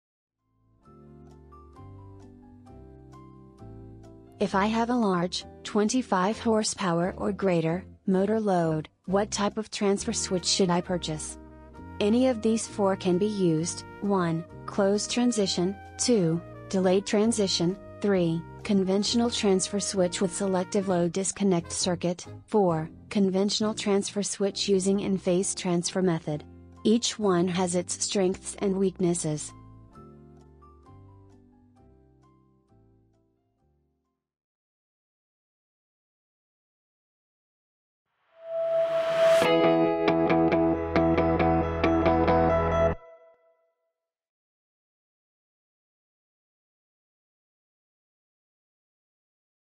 TechXpert: Which type of transfer switch will fit a larger motor load up to 25 HP or greater?
Which type of transfer switch will fit a larger motor load up to 25 HP or greater? Is a closed transition transfer switch the right option for a 25 HP motor load?